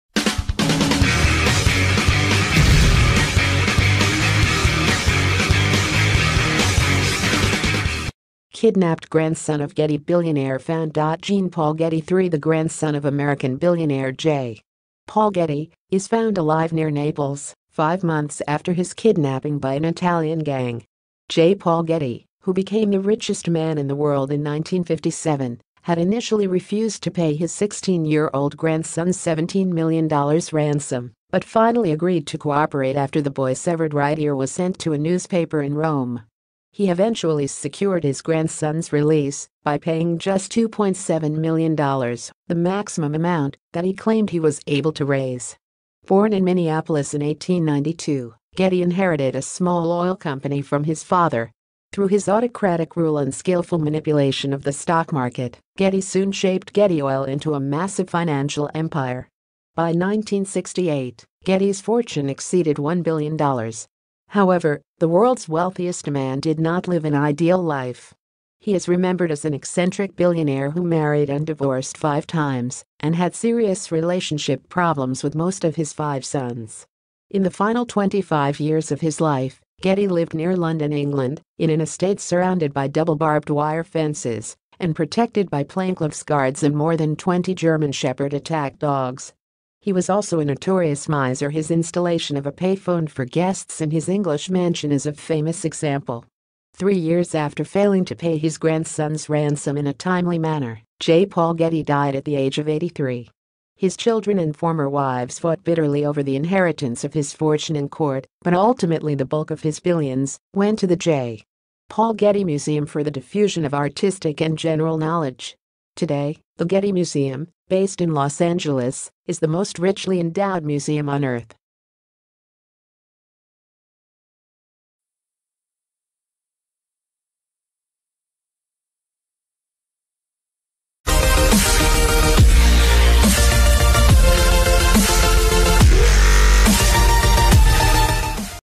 Kidnapped grandson of Getty billionaire found
Jean Paul Getty III, the grandson of American billionaire J. Paul Getty, is found alive near Naples, five months after his kidnapping by an Italian gang. J. Paul Getty, who became the richest man in the world in 1957, had initially refused to pay his 16-year-old grandson’s $17 million ransom but finally agreed to cooperate after the boy’s severed right ear was sent to a newspaper in Rome. He eventually secured his grandson’s release by paying just $2.7 million, the maximum amount that he claimed he was able to raise.

Born in Minneapolis in 1892, Getty inherited a small oil company from his father. Through his autocratic rule and skillful manipulation of the stock market, Getty soon shaped Getty Oil into a massive financial empire. By 1968, Getty’s fortune exceeded $1 billion. However, the world’s wealthiest man did not live an ideal life. He is remembered as an eccentric billionaire who married and divorced five times and had serious relationship problems with most of his five sons.

In the final 25 years of his life, Getty lived near London, England, in an estate surrounded by double barbed-wire fences and protected by plainclothes guards and more than 20 German shepherd attack dogs. He was also a notorious miser–his installation of a payphone for guests in his English mansion is a famous example. Three years after failing to pay his grandson’s ransom in a timely manner, J. Paul Getty died at the age of 83.

His children and former wives fought bitterly over the inheritance of his fortune in court, but ultimately the bulk of his billions went to the J. Paul Getty Museum “for the diffusion of artistic and general knowledge.” Today, the Getty Museum, based in Los Angeles, is the most richly endowed museum on earth.